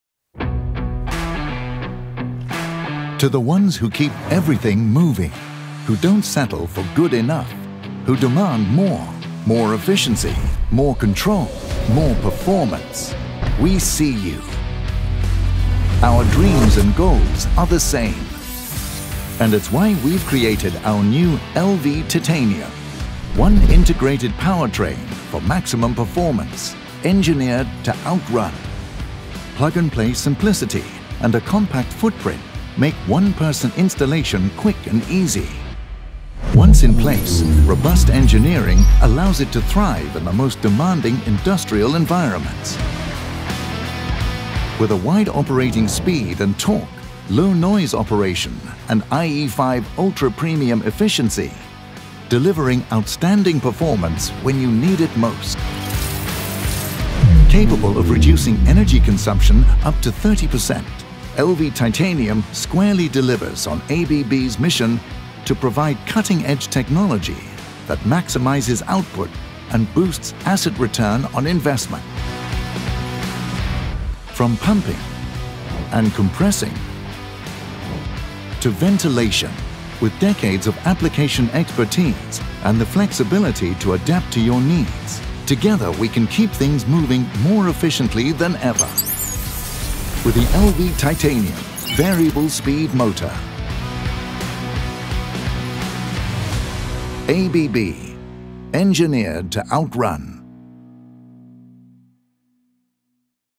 Meet ABB's LV Titanium variable speed motor: The Future of Industrial Powertrain Performance
Demand more from every component. Introducing ABB's LV Titanium variable speed motor: an integrated powertrain built for maximum performance, ultra efficiency, and simplified installation.

Engineered to outrun the competition, this plug-and-play solution is designed for one-person setup and thrives in the toughest industrial environments. With IE5 ultra-premium efficiency, low noise operation, and the ability to cut energy consumption by up to 30%, the LV Titanium helps you boost performance and ROI — whether you're pumping, compressing, or ventilating.

With decades of ABB expertise behind it, LV Titanium is ready to move your operation forward — faster, smarter, and more efficiently.

ABB: Engineered to outrun.